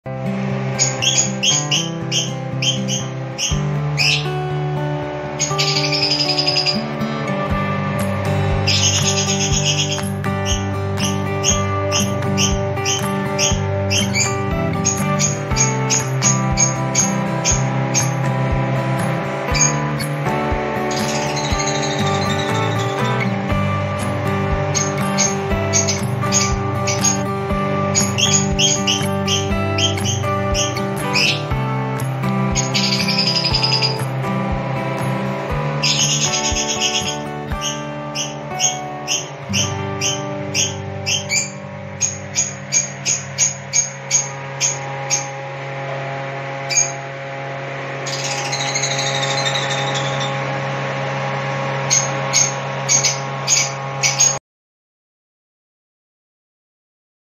Ring mutation love birds for sale || 2, mav parblue /1, violet fisher split parblue || only 3k 👌❣
Parents, violet parblue male, mav fisher female
Mav parblue 2 piece, 1 piece violet fisher split parblue
3 piece 3000 only
Anand Gujarat

Hello dosto
Le aye ek or love birds ring mutation ki bahot hi acchi quality me active and healthy ji ha mav parblue ka 2 piece 1 piece violet fisher split parblue Jis kisi bhai ko chahiye ho wo screen par diye Gaye number par contact kare ☝